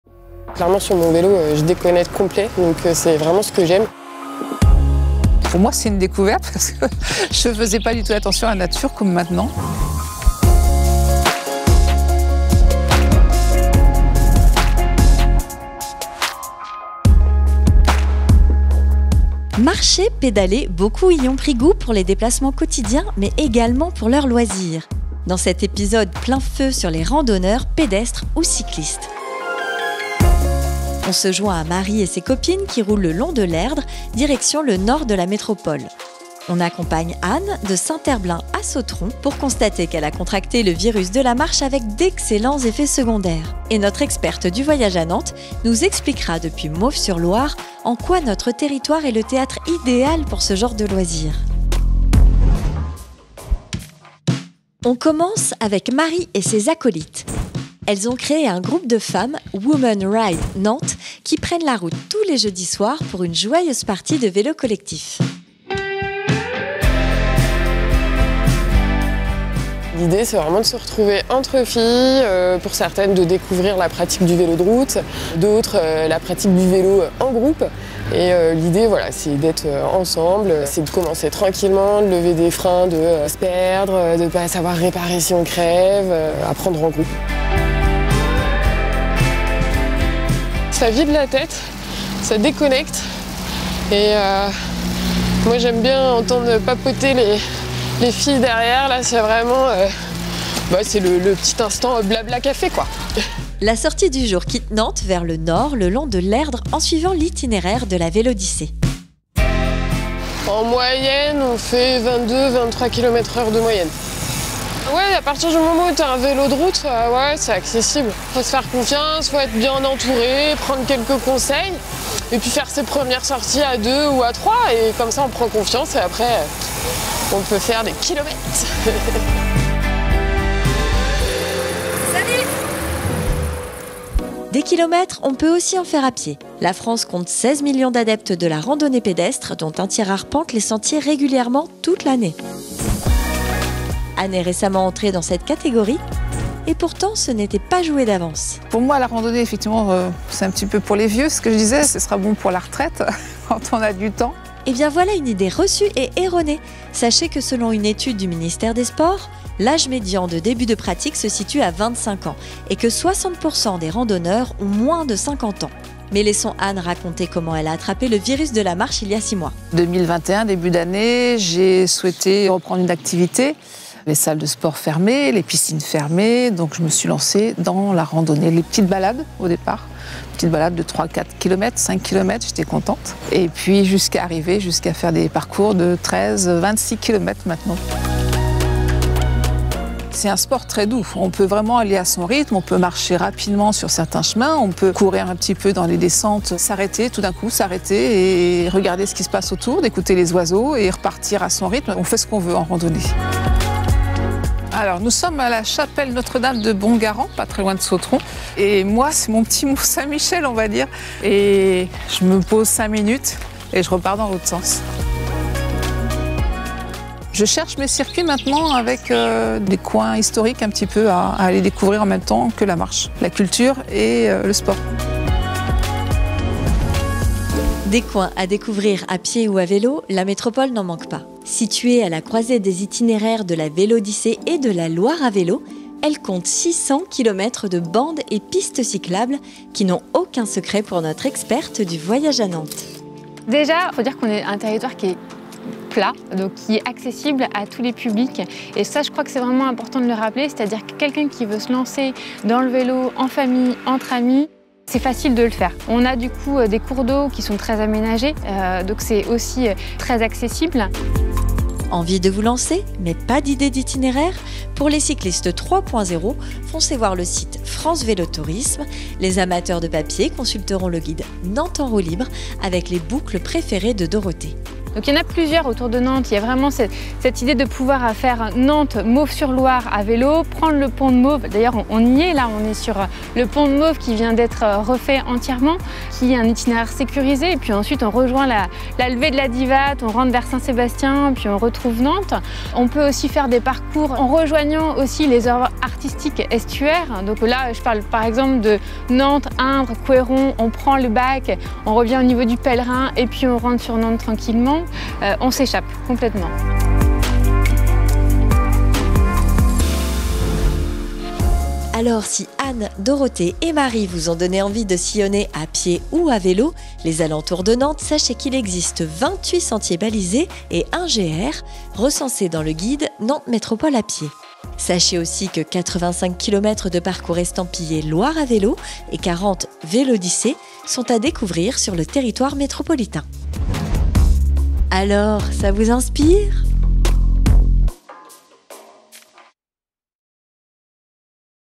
Nantes Inspirante #4 Rando ou vélo, leur nouvelle passion du week-end
Retour à la nature, aspiration au bien-être et à la santé, dépaysement, goût pour le tourisme de proximité… La randonnée a le vent en poupe. Elle se pratique d’abord à pied : environ 16 millions de randonneurs français qui arpentent les sentiers. La rando cyclo a aussi ses adeptes, avec le développement de parcours vélo favorisant l’itinérance (Loire à vélo, Vélodyssée…). Dans cet épisode, la parole est donnée à des habitantes de la métropole qui se sont récemment converties à l’une ou l’autre de ces disciplines. Pilote du guide « Nantes en roues libres ! », récemment édité par le Voyage à Nantes, notre experte explique en quoi la métropole nantaise et ses environs sont particulièrement propices à la pratique du vélo en toute liberté.